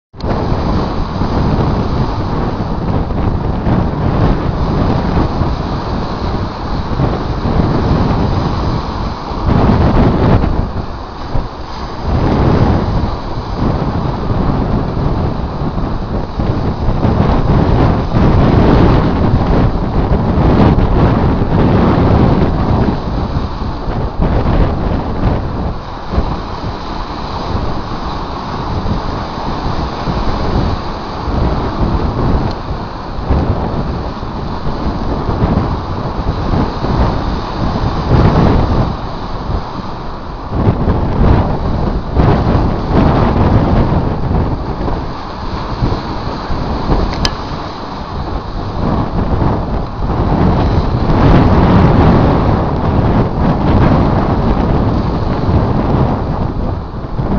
Lake Michigan Zen 80508 3/3
Looking south towards the Loop, not a particularly windy day by Chicago standards.  Absorb the sound and sights and find peace.